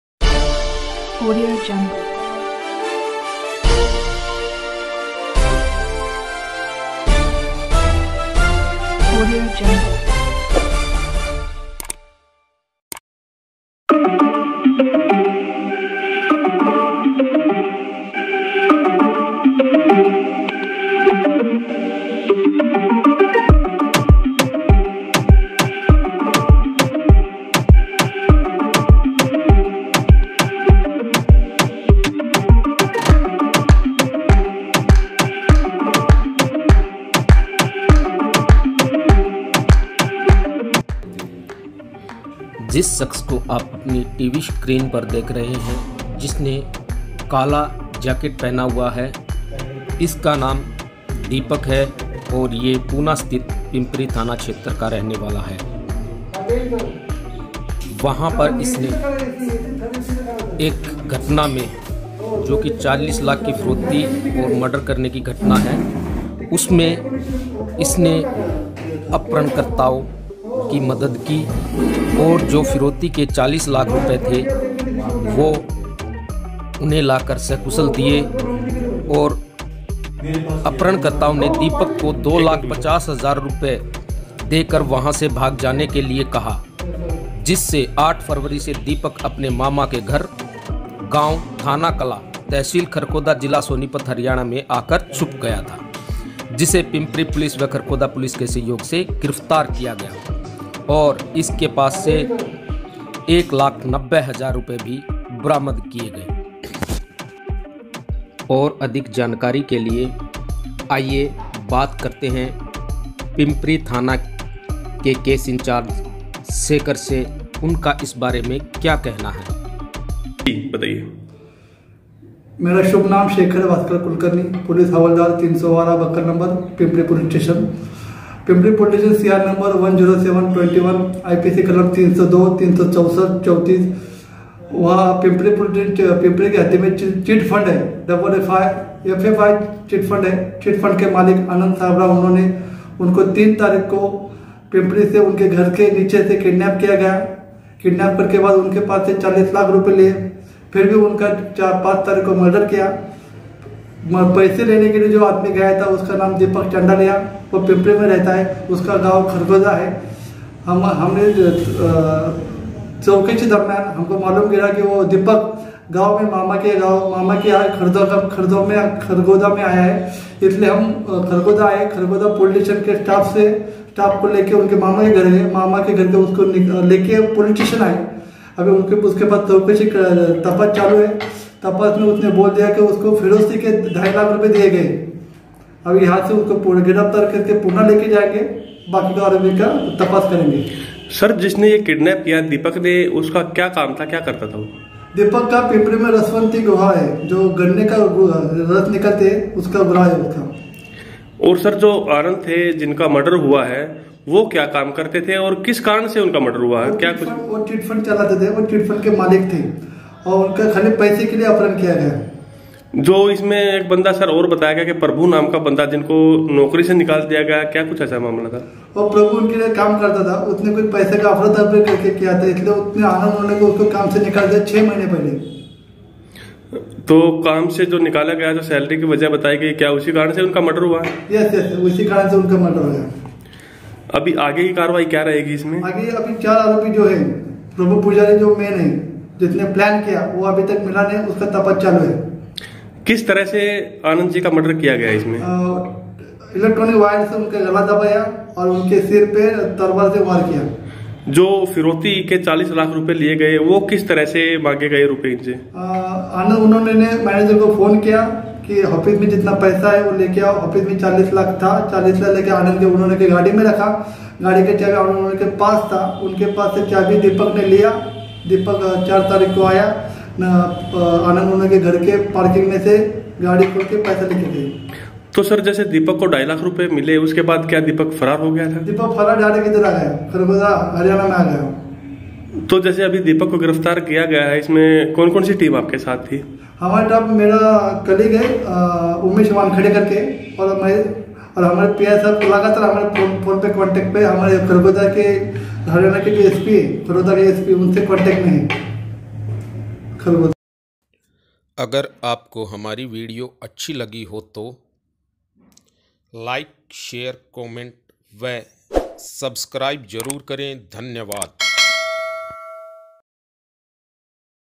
40 लाख की फिरौती मांगने व मर्डर मामले में एक आरोपी गिरफ्तार।
महाराष्ट्र के पूना स्थित पिंपरी थाना के केस इंचार्ज शेखर ने जानकारी देते हुये बताया कि 3 फरवरी को एफएफआई चिट फंड कंपनी के मालिक आनंद साहिब राव का अपहरण उसके घर के पास से हुआ जिसमें कंपनी के मालिक आनंद साहिब राव के फ़ोन से ही चिट फंड कंपनी के मैनेजर को फ़ोन 40 लाख रुपए की फिरौती के लिये फ़ोन करवाकर कहा गया कि उसकी गाड़ी में 40 लाख रुपये रख दिये जायें जिसमे सारे रुपए ड्राइवर के द्वारा उसकी गाड़ी में रखवा दिये गये। जिसमे अपरहण कर्ता ने पिंपरी के रहने वाले दीपक उम्र 34 जोकि  गन्ने से जूस निकालने का काम करता था उसको चालीस लाख रुपये लाने के लिये भेजा गया और दीपक ने वो काम बखूभी निभाया और सारे रुपये लाकर अपरहण कर्ता को दे दिये जिस काम के दीपक को 2 लाख 50 हजार रुपये भी मिले और उसे वहाँ से भाग जाने के लिए भी कहा गया जिसमें 8 फरवरी से दीपक अपने मामा के घर गांव थाना कलां तहसील खरखौदा जिला सोनीपत हरियाणा में आकर छुप गया था। जिसे पिंपरी पुलिस व खरखौदा पुलिस के सहयोग से गिरफ्तार कर लिया गया।

केस इंचार्ज शेखर ने ये भी बताया कि इस अपहरण व मर्डर के कुल 5 आरोपी है जिनके नाम बाबू, पप्पू, सागर और प्रभु है।
प्रभु नाम का व्यक्ति चिट फंड कंपनी में ही नोकरी करता था जिसका सैलरी बढ़ाने को लेकर कंपनी मालिक आनंद साहिब राव के साथ झगड़ा हुआ था जिसे 6 महीने पहले कंपनी से निकाल दिया गया था। जिसको लेकर प्रभु ने अपने साथी बाबू, पप्पू, सागर
 के साथ मिलकर एफ एफ आई कंपनी के मालिक का उसके घर के पास से अपहरण कर फिरौती की मांग की और डिमांड किये हुये 40 लाख रुपये लेकर बिजली की तार से गला दबाकर आनंद की हत्या की गई और तेज धारदार तलवार से उसके सिर पर भी कई वार किये गये। जिसके आरोप में रविवार को एक आरोपी को खरखौदा से गिरफ्तार किया गया है।

केस इंचार्ज के अनुसार दीपक से 190000- एक लाख नब्बे हजार रुपये भी बरामद कर लिये गये है।